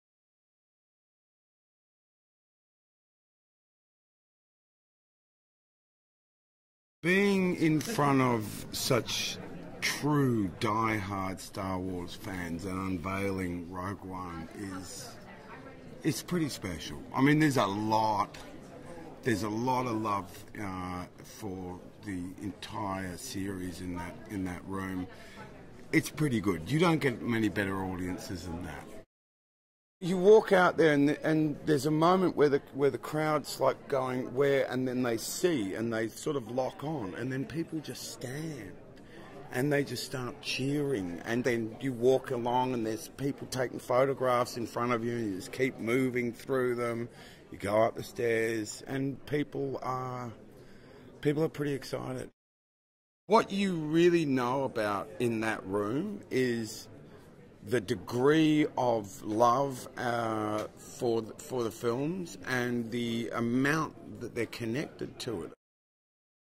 Star Wars celebration panel soundbite Ben Mendelsohn
Ben Mendelsohn at the 2016 Star Wars celebration